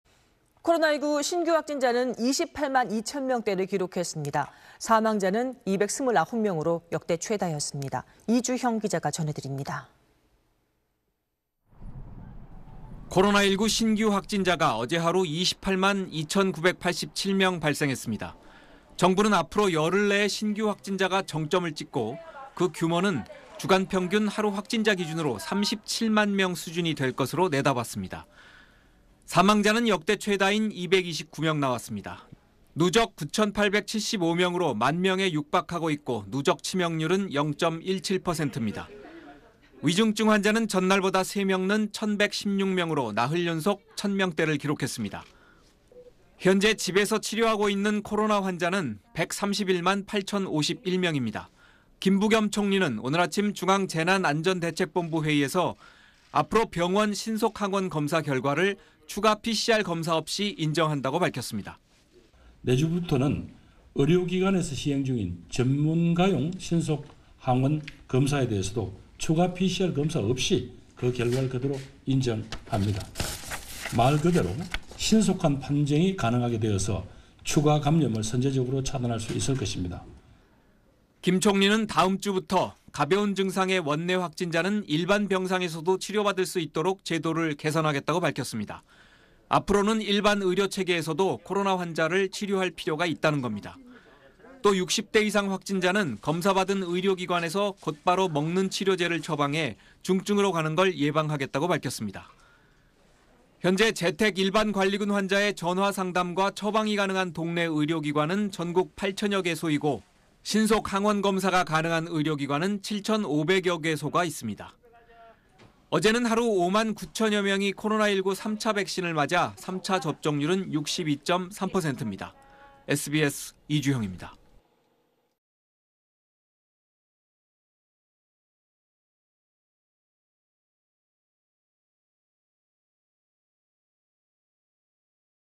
신규 확진 28만 명대…"전문가용 신속항원 양성도 인정" / SBS
코로나19 신규 확진자는 28만 2천 명대를 기록했습니다. 사망자는 229명으로 역대 최다였습니다.
이주형 기자가 전해드립니다.